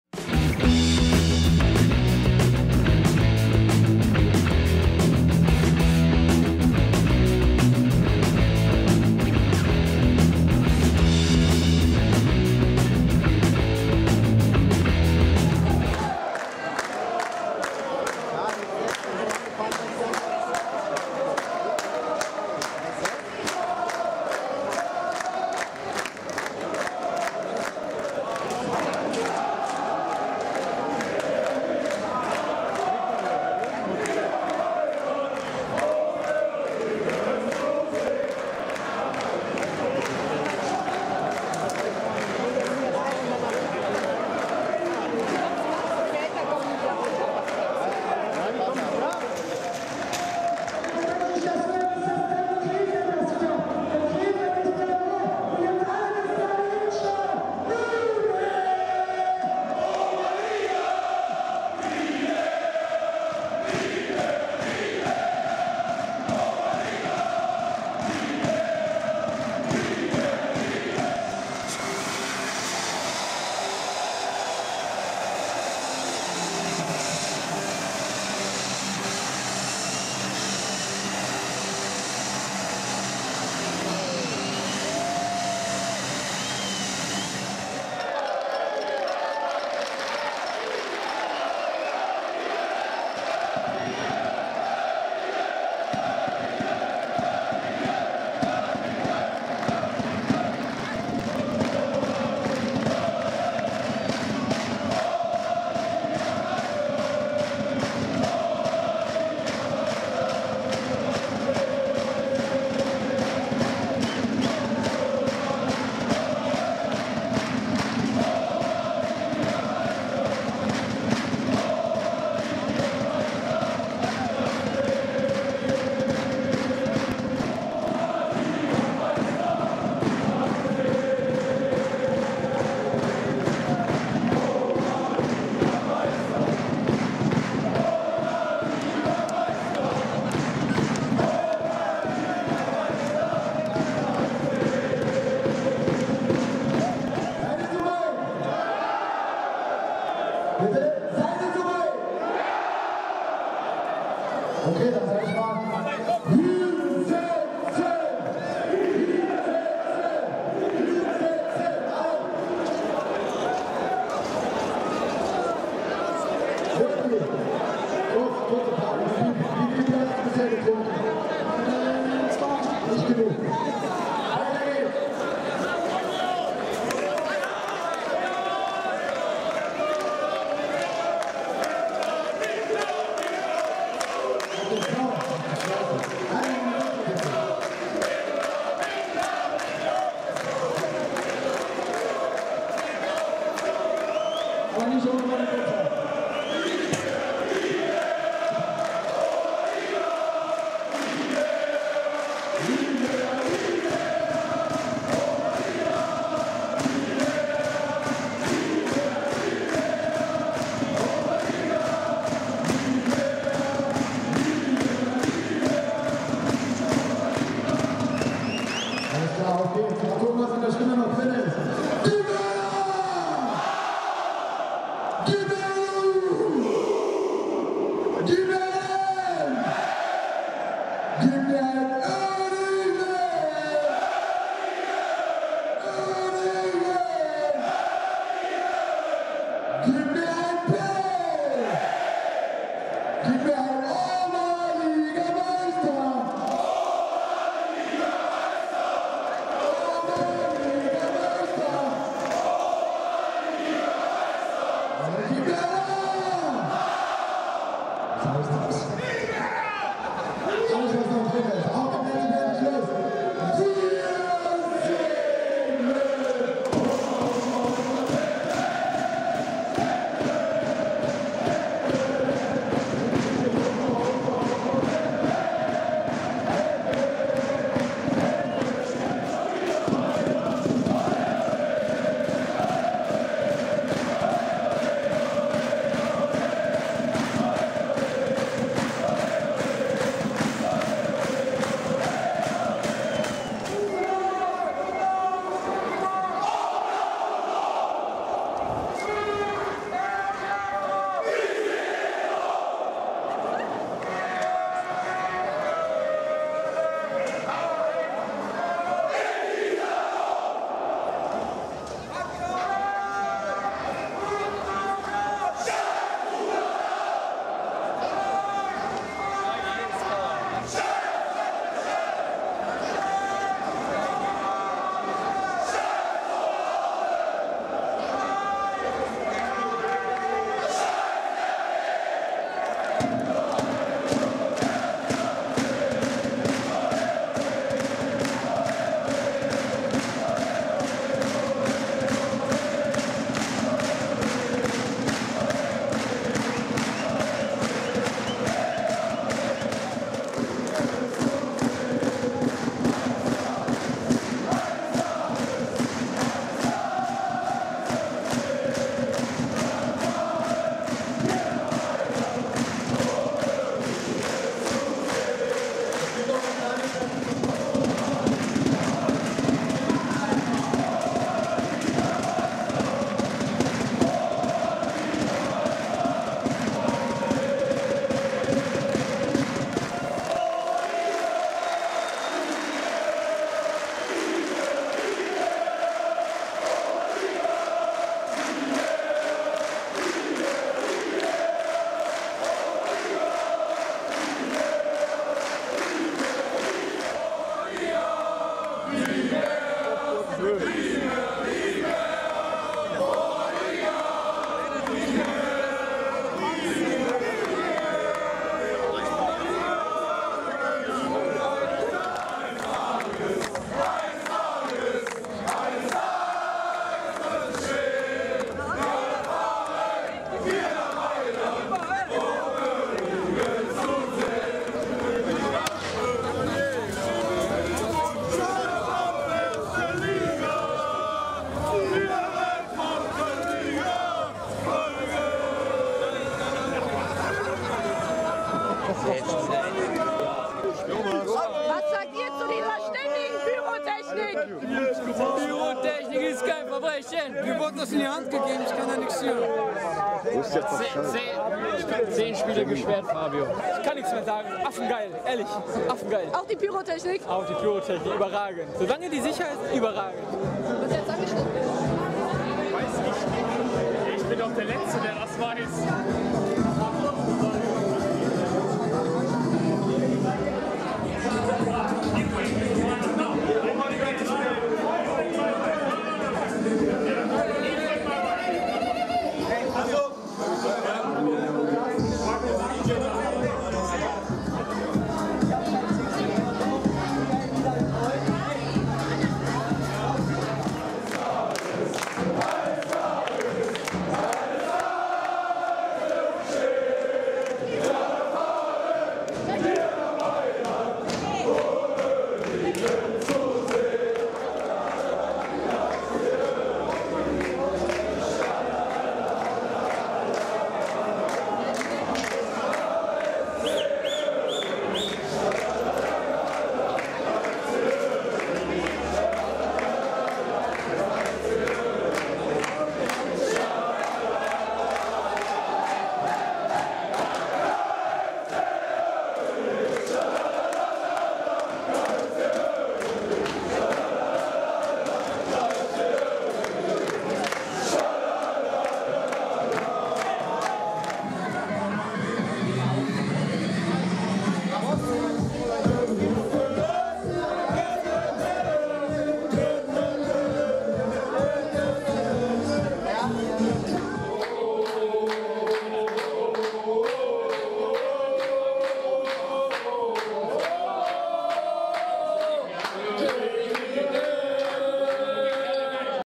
Uerdingerblock Aufstiegsfeier
Zusammenschnitt der Aufstiegsfeier des KFC Uerdingen nach dem 0:4 in Hiesfeld